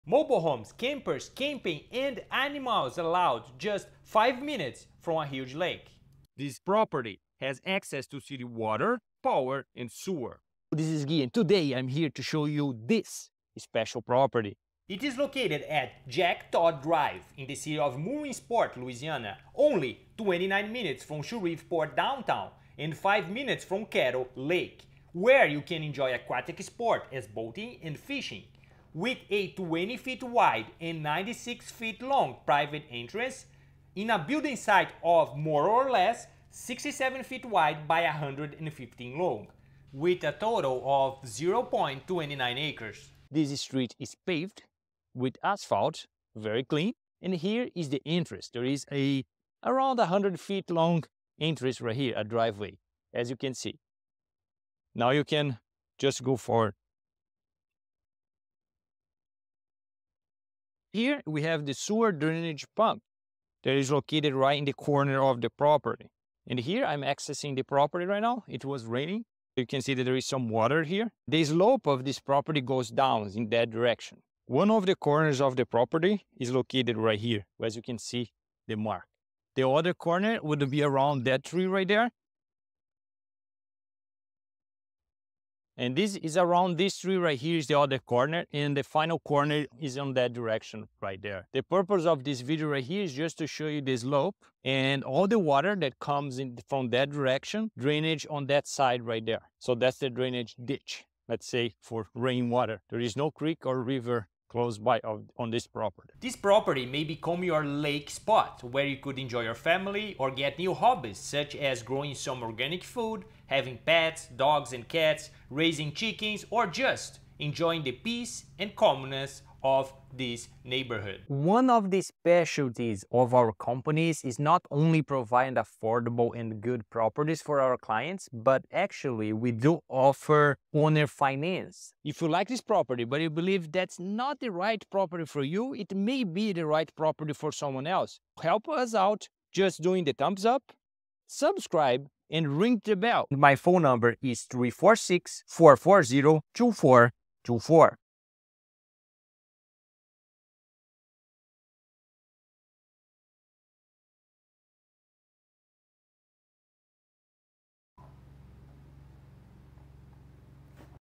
Land For Mobile Home 5 Min From A Huge Lake
PRICE: $15,500

PROPERTY DETAILS
Location: Ref. Point 7930 Jack Todd Dr, Mooringsport, LA 71060
Status: Available
State: Louisiana
County: Caddo Parish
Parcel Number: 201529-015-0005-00
Road Access: Yes - Paved
Electricity: Yes
City water: Yes
City sewer: Yes
Terrain: slight slope